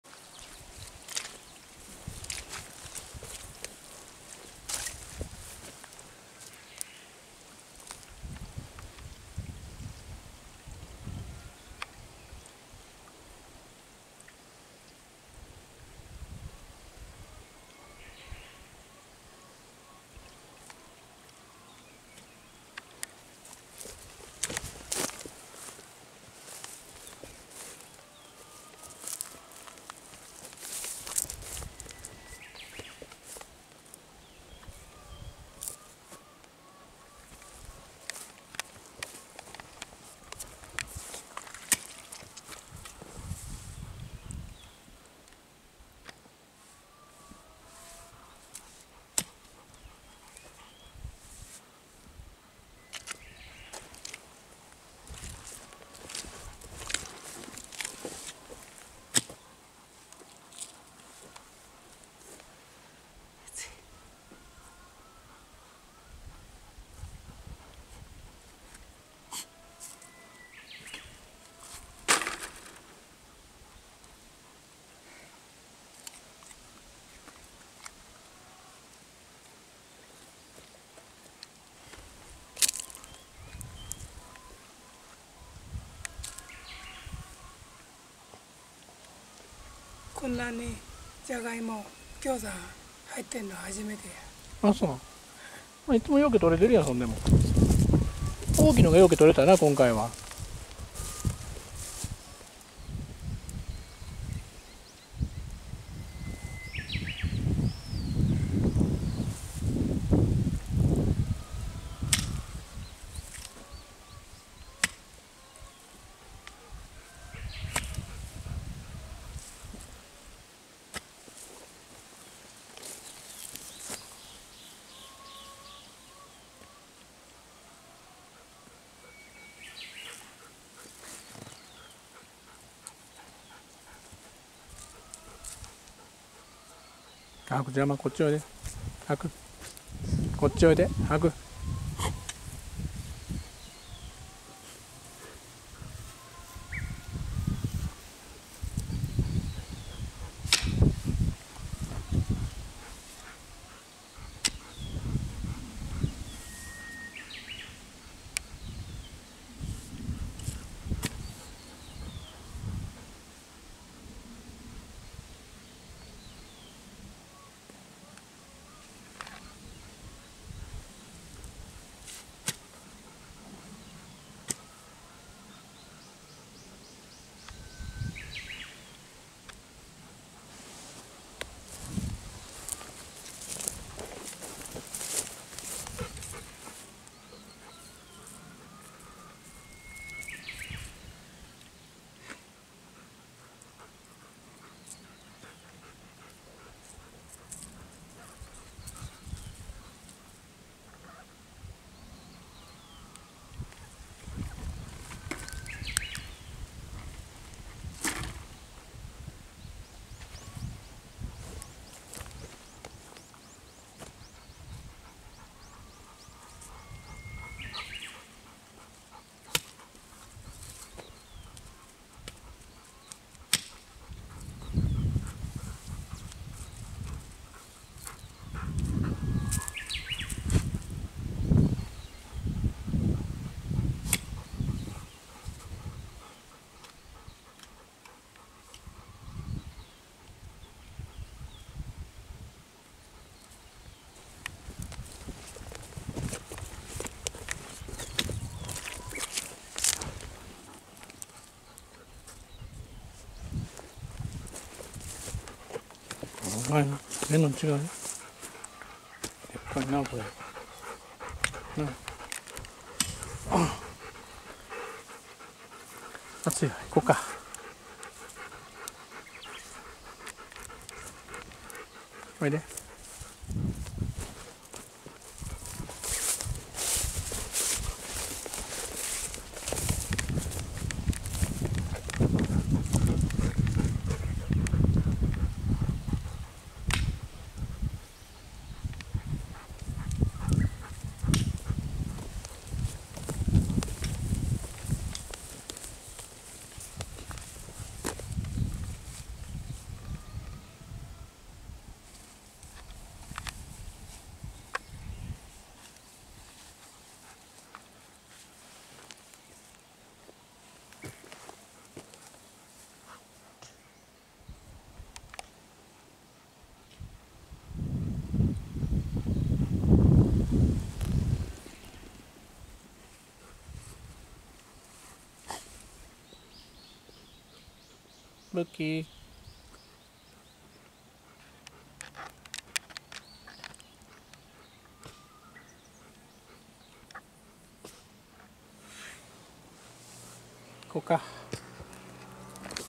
暑い昼にジイチャンとゴールデンレトリバーのジャガイモ畑
休憩の終わったジイチャンはジャガイモを掘り始めます。
去年は猪にジャガイモ畑を荒らされて、全ての努力が無駄になりましたが、今年は電気柵のおかげか、被害は全くなしで今日を迎えました。
かなり以前から私は一株づつ掘って食べる分を自宅に持ち帰っていましたが、今日は全て掘り上げる予定です。
ジャガイモも上手く保存すれば、来年の春まで保存できるので、助かります。